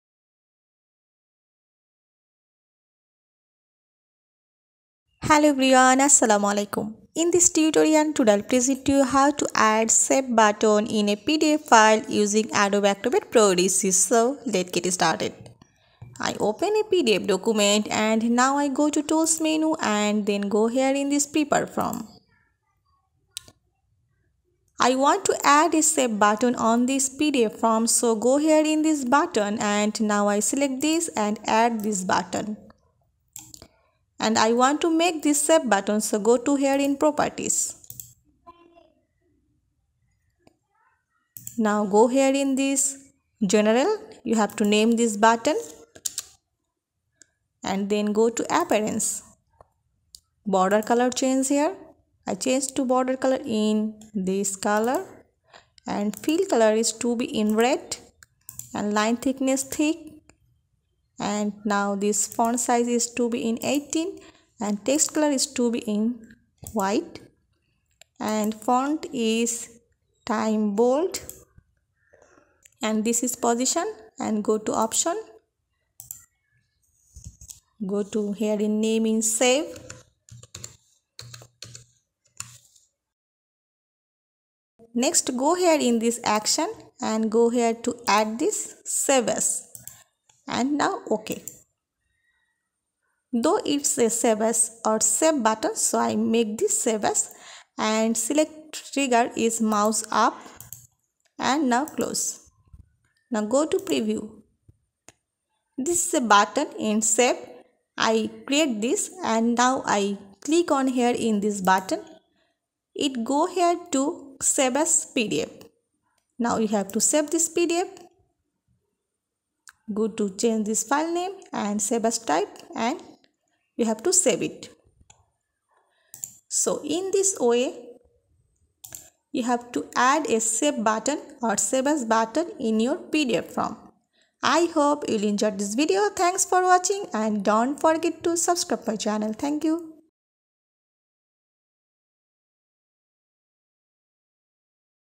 How to add a save button in a pdf file with Adobe Acrobat Pro DC
Hello everyone, Today I will present you, How to add a save button in a pdf file with Adobe Acrobat Pro DC.